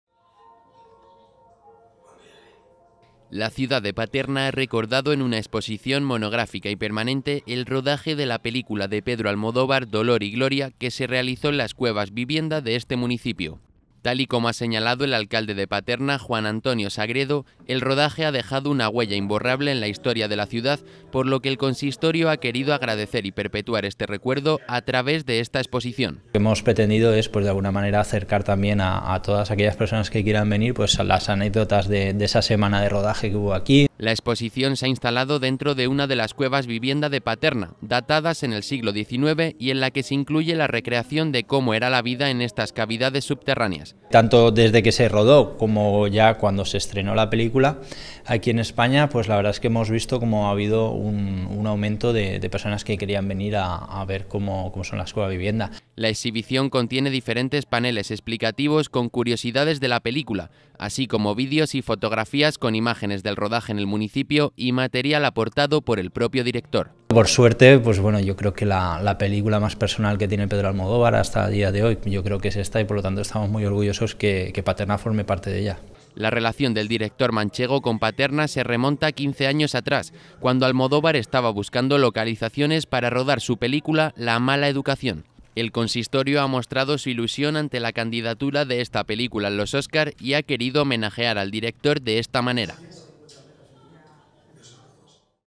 Paterna recuerda el rodaje de la película de Almodóvar, 'Dolor y gloria'
La ciudad de Paterna ha recordado en una exposición monográfica y permanente el rodaje de la película de Pedro Almodóvar 'Dolor y gloria' que se realizó en las cuevas vivienda de este municipio.